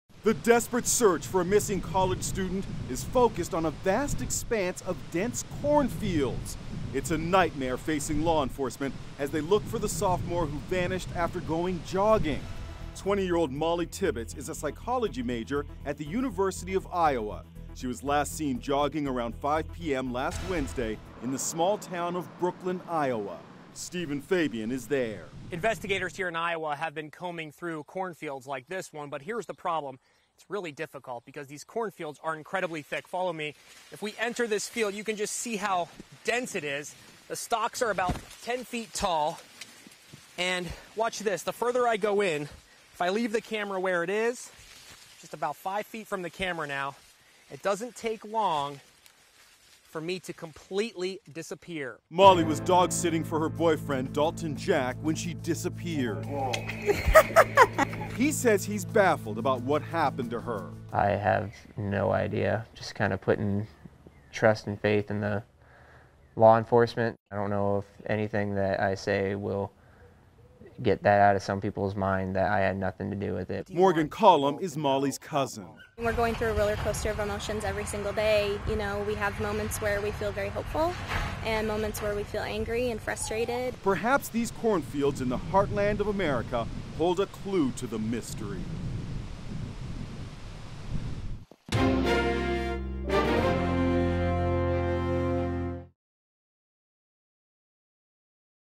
Search for Missing Iowa Woman Mollie Tibbetts Expands to Cornfields
The desperate search for a missing Iowa college student is focused on a vast expanse of dense cornfields. It's a nightmare facing law enforcement as they look for the sophomore who vanished after going jogging last week. Mollie Tibbetts, 20, is a psychology major at the University of Iowa. She was last seen jogging on Wednesday in the small town of Brooklyn, Iowa. Investigators have been combing through cornfields but they are incredibly thick.